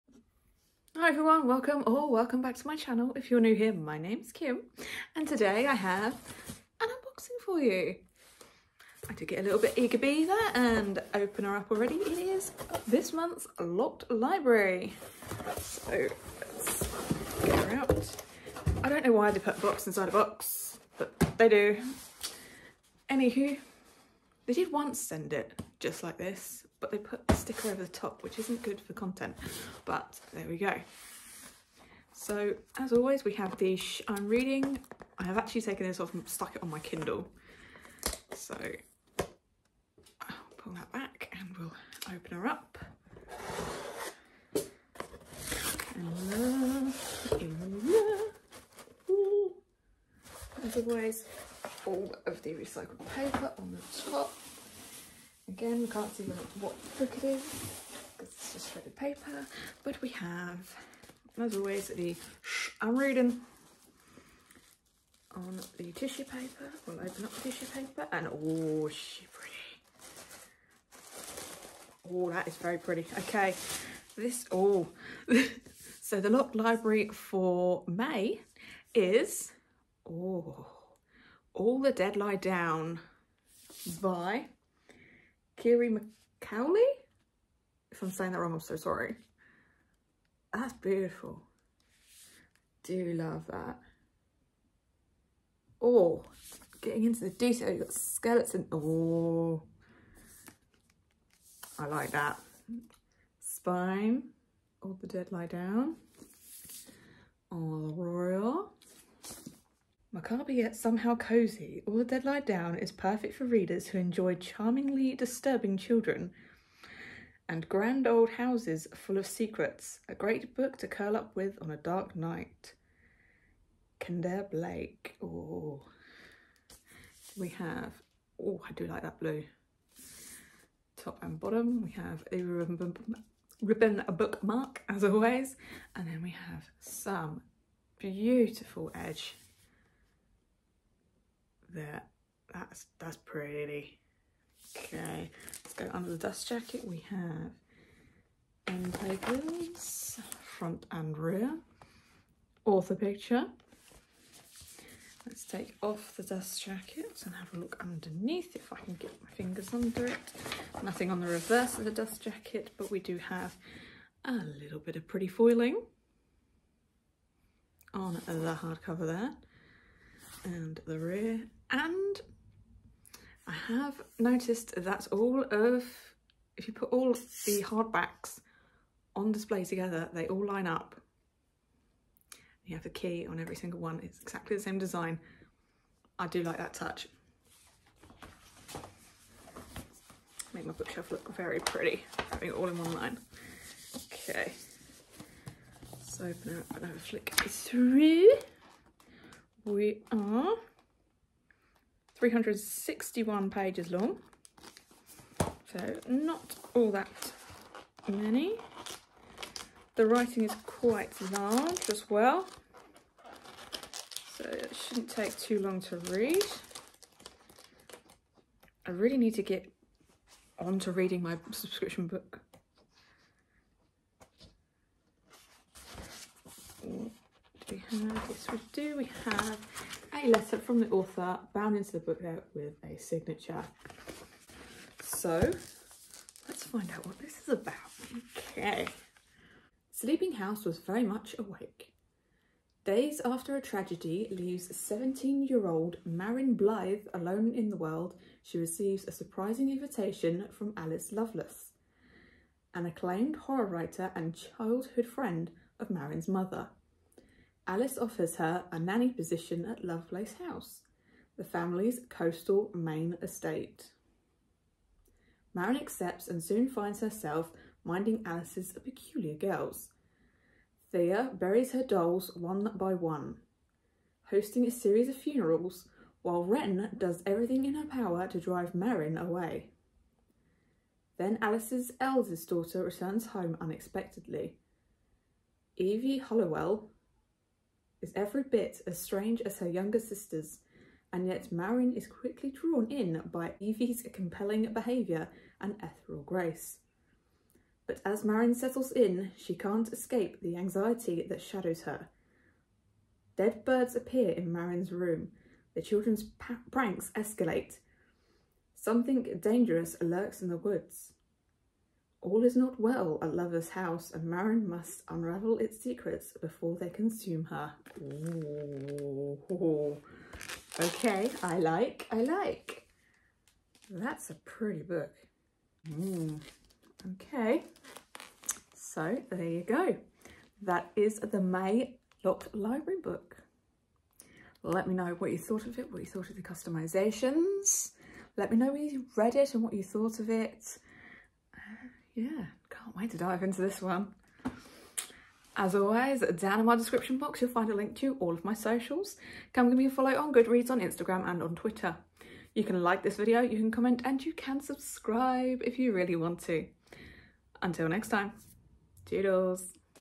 May Locked Library Unboxing
Watch me unbox May’s Locked Library for the theme ‘Haunted Summer’.

📚Get a 60 days trial of Scribd using my code and I’ll get 30 days free too.